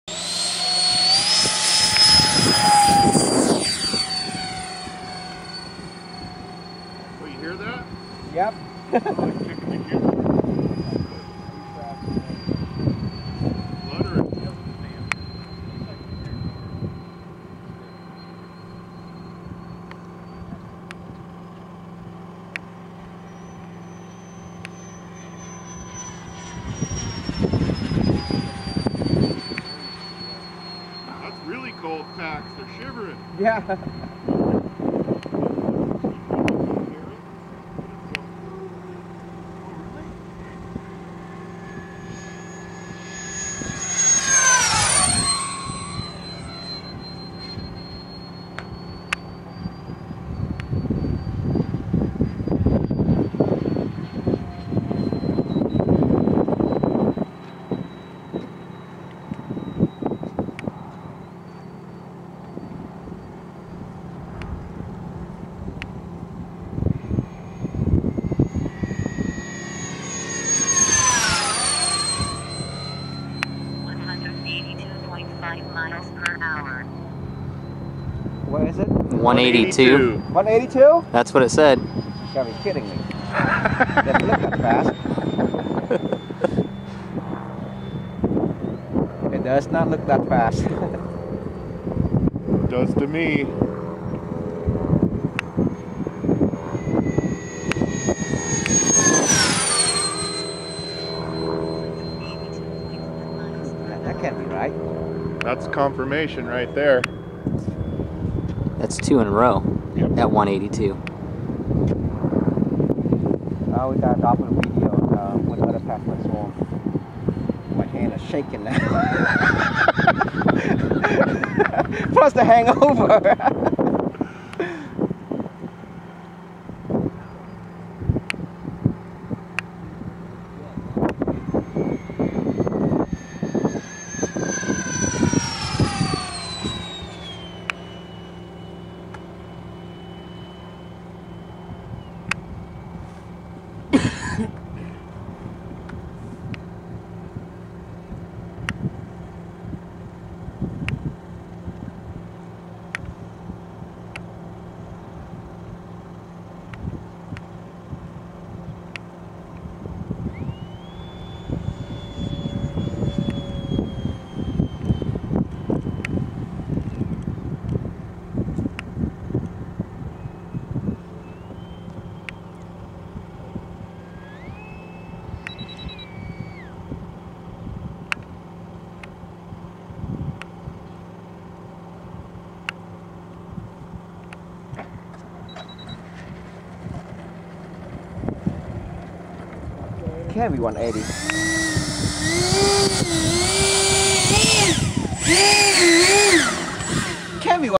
Habu 32 Power by TJ80 12s
E-Flite Habu 32, power by TJ80, NeuMotor 1512 2Y, Castle HV120, 12s 3300mAh.
AUW 8 lbs.